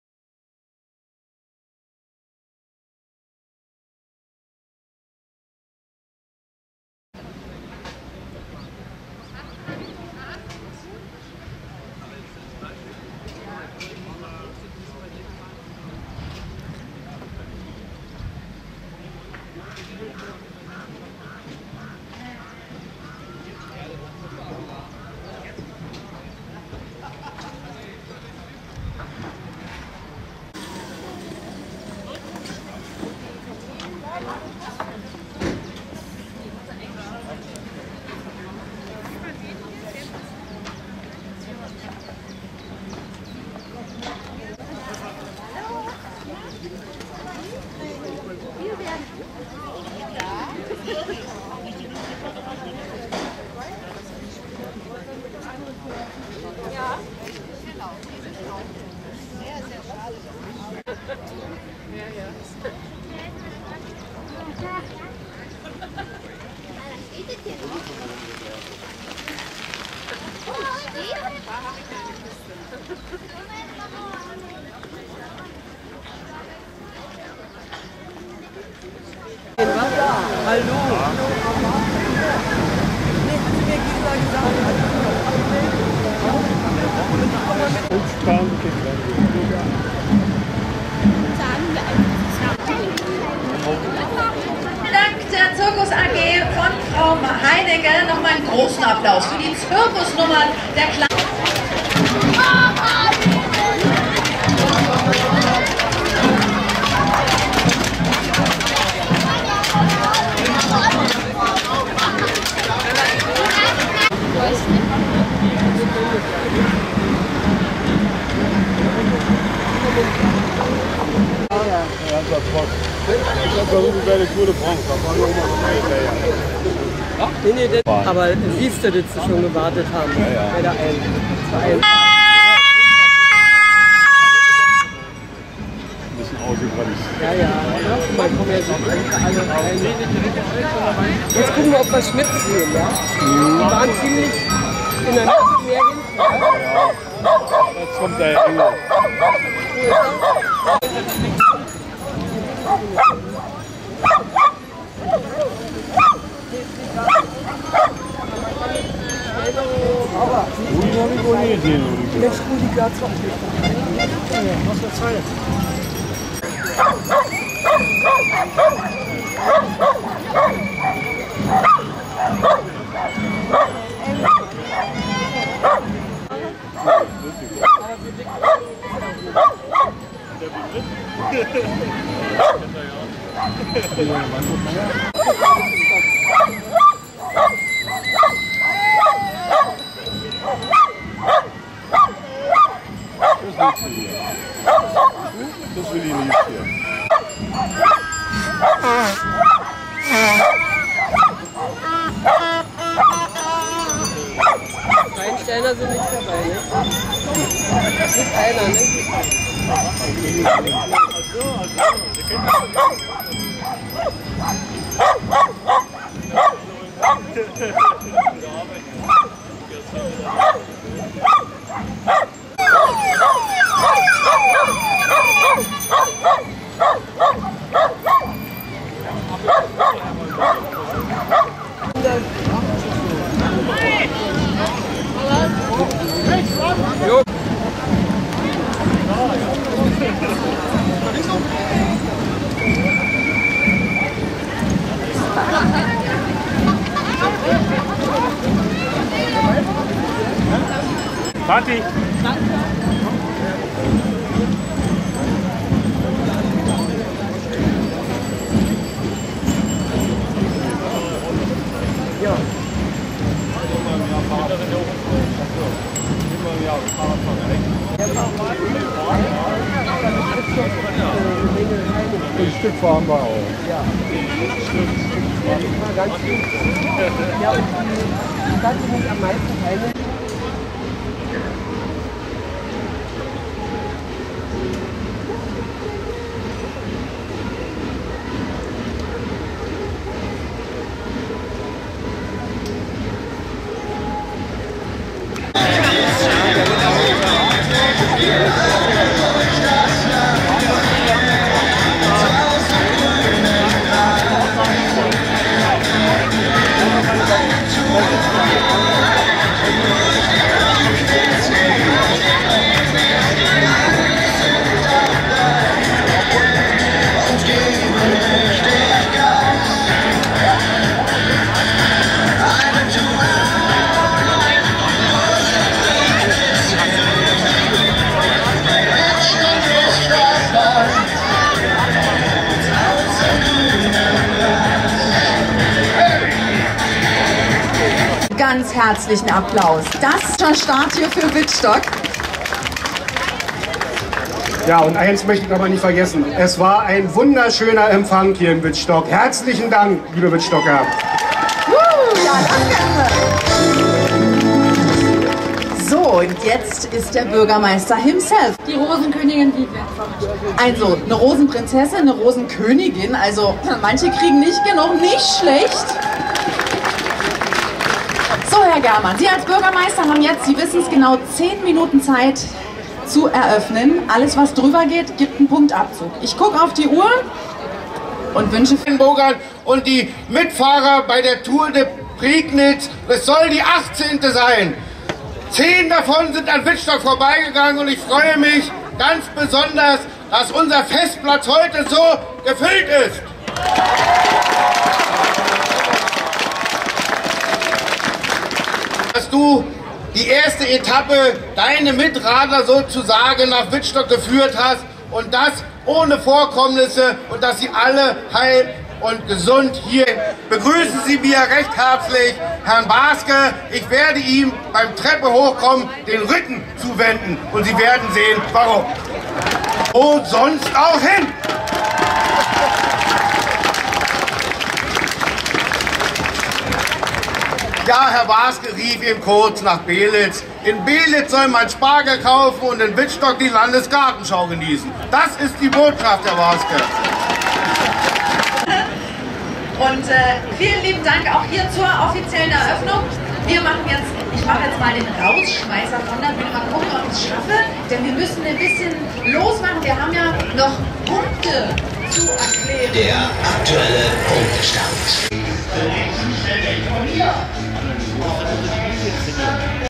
Tour de Prignitz 2015 - Ziel Wittstock
Beschreibung
Eine Radtour durch die Prignitz für jedermann